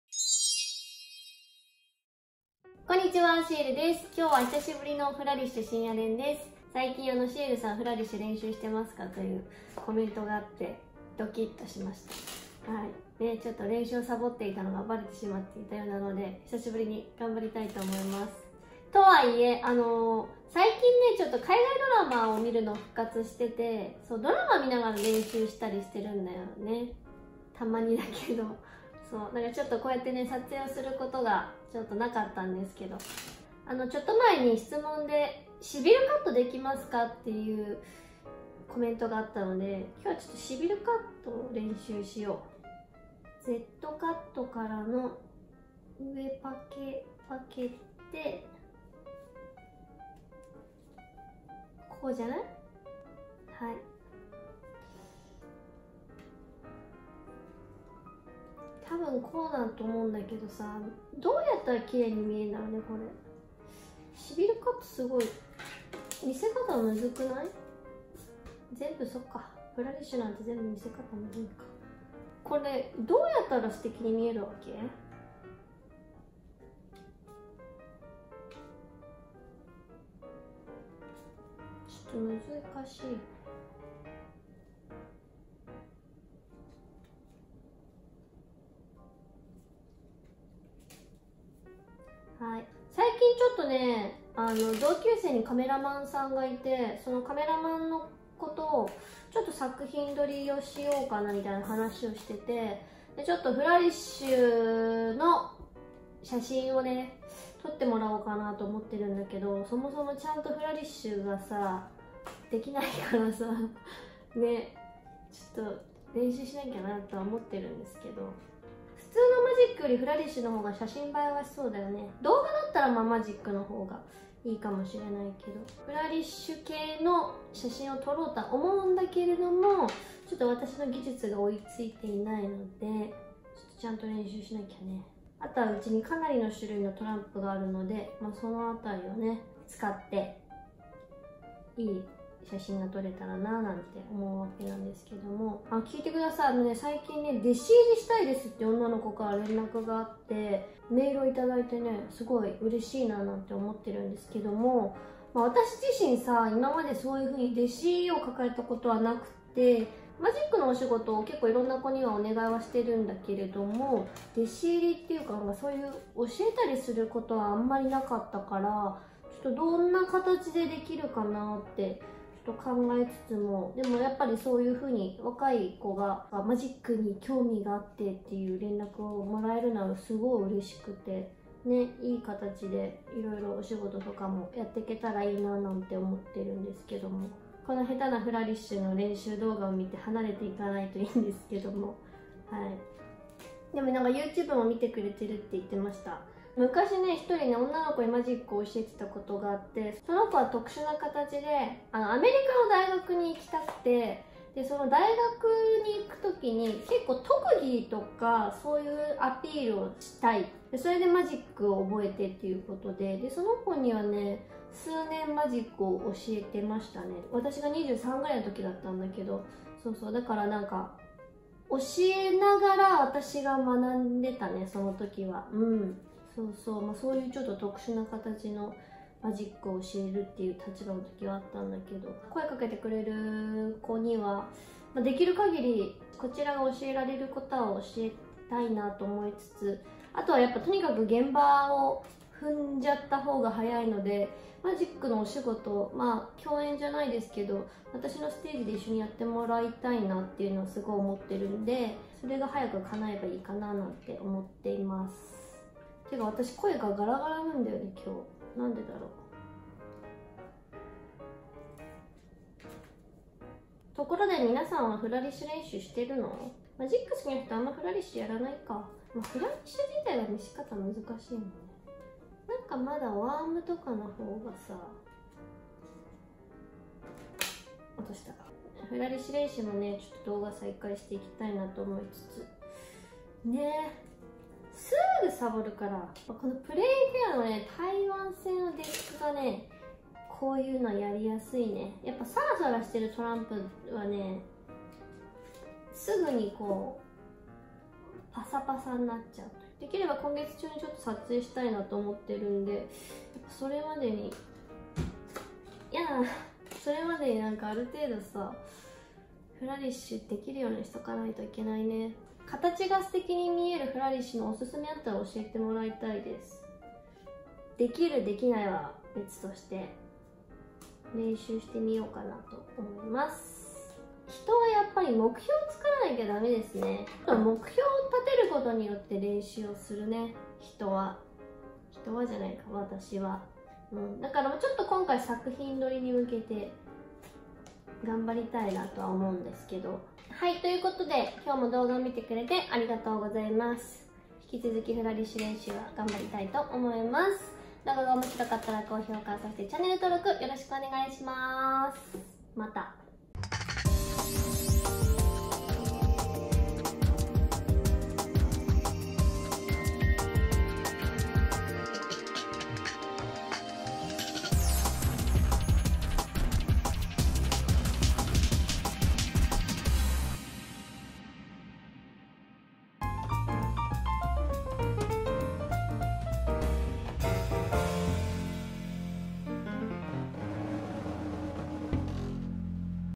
【マジシャンになりたい】マジシャンシエルが語ります【Cardistry Girl】
久しぶりのフラリッシュ深夜練と共に
近況について語る。
どう？みんなもフラリッシュしてる？？？